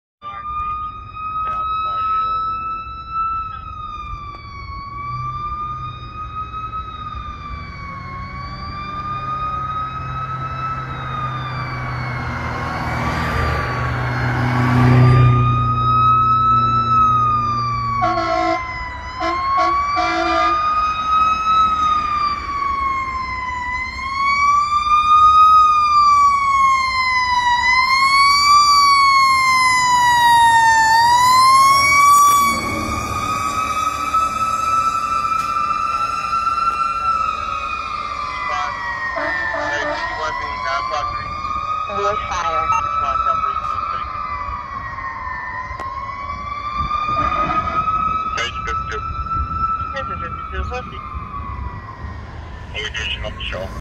*** SCREAMING Q*** PFD Water Tower 38 Responding
Philadelphia Fire Department Water Tower 38 Responding to a reported Dwelling Fire

Water Tower 38 2012 KME Severe Service Predator MFD  1500/500 50' Snozzle